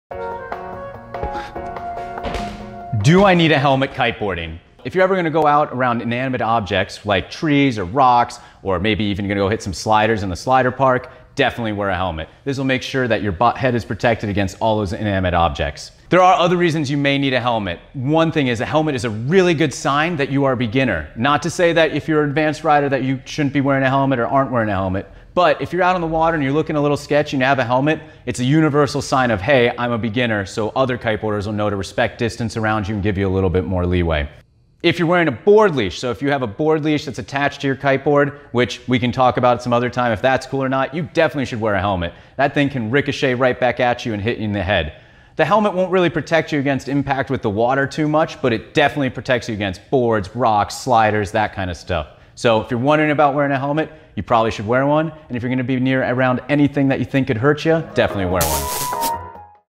Should I wear a helmet when kiteboarding? Kook Academy: Liquid Force Kiteboarding
To wear a helmet or not wear a helmet, that is the question.  Brand Manger Rich Sabo walks you through his thoughts and guidance on the issue in this edition of Kook Academy.  Long story short, if you think you need a helmet - you should probably wear one!

We welcome comments and suggestions as we love to hear from you.